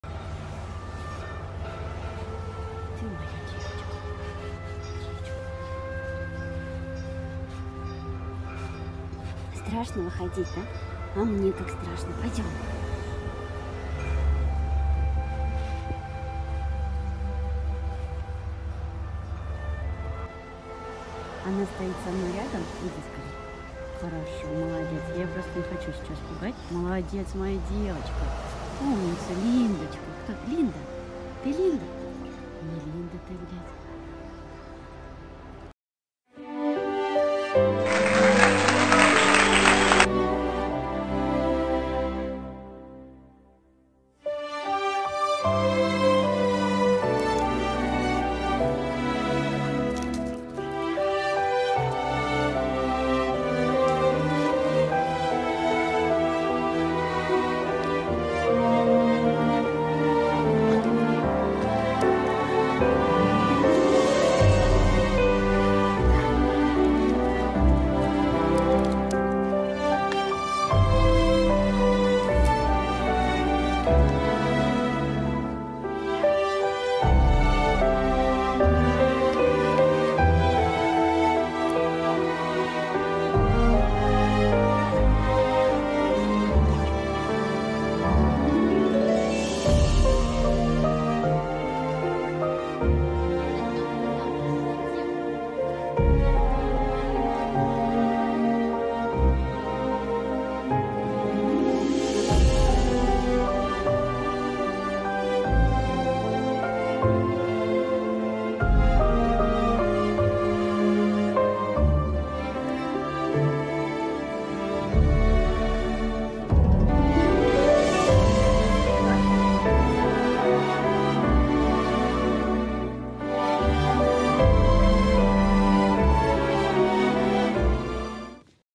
Наоми дома! Счастливая История!
❗️❗️❗️Счастливая История ❗️❗️

Да-да, Наоми-Гнома!!! Вот кто бы знал тогда?? Год назад))! Как все сложится! Вопреки всем сложностям! Она растопила сердце 💔 одной Девочки на второго Корсика)))

Спасибо всем за это чудо!!! Пусть счастье их будет обоюдно-безмерным!